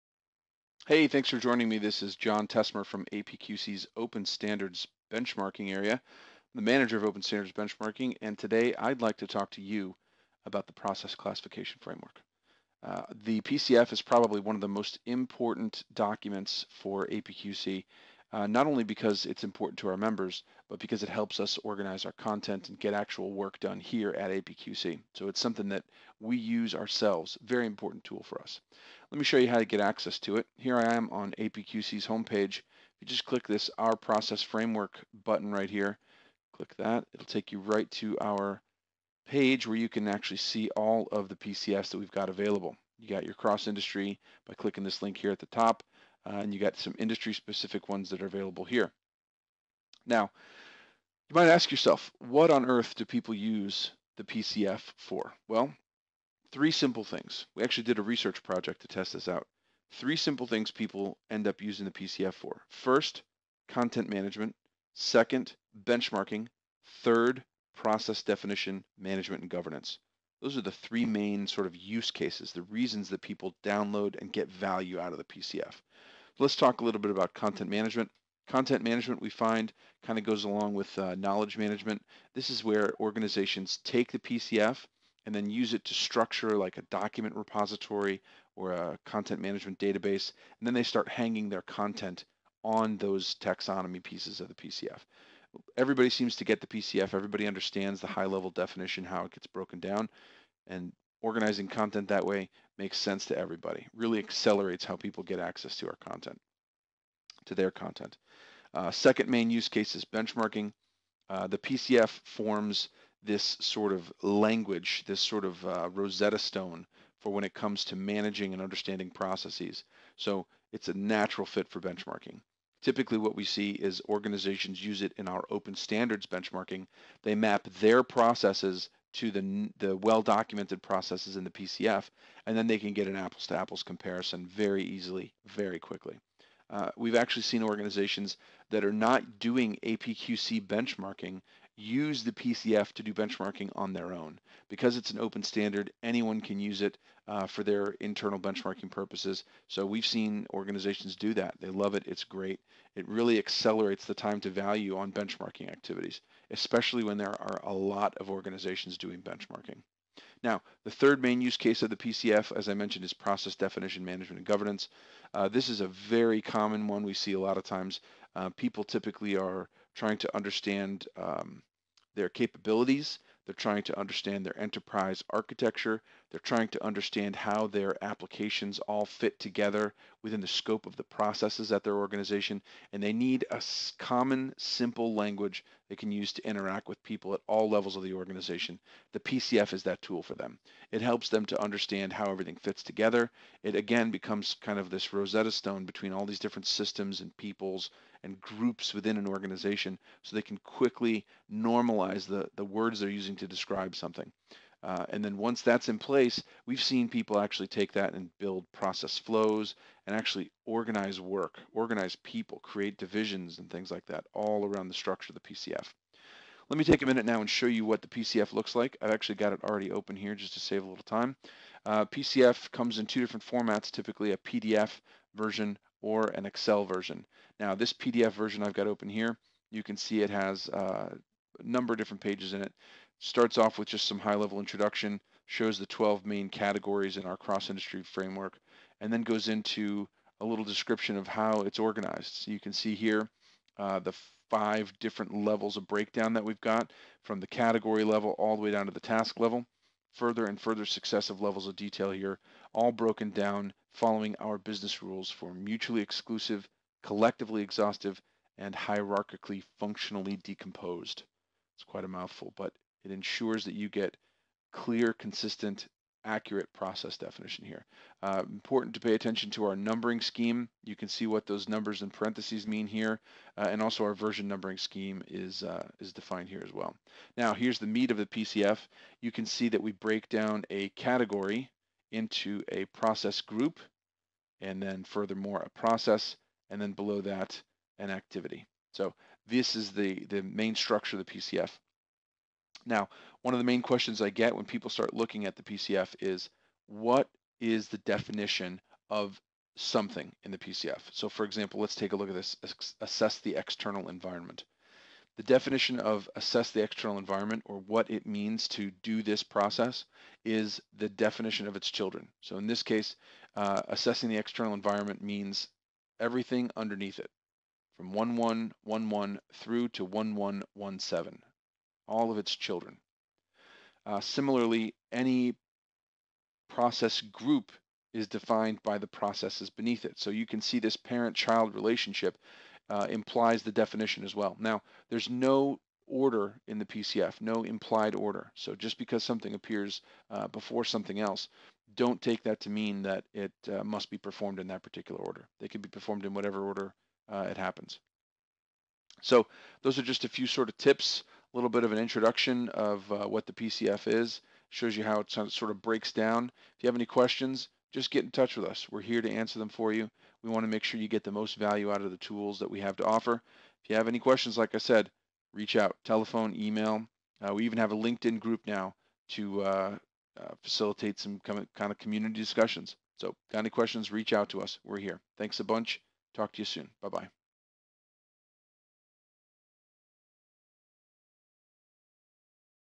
Understanding Critical Pieces Of The Process Classification Framework (PCF)
How to understand the PCF numbering scheme, hierarchical structure, and why understanding the definitions are critical.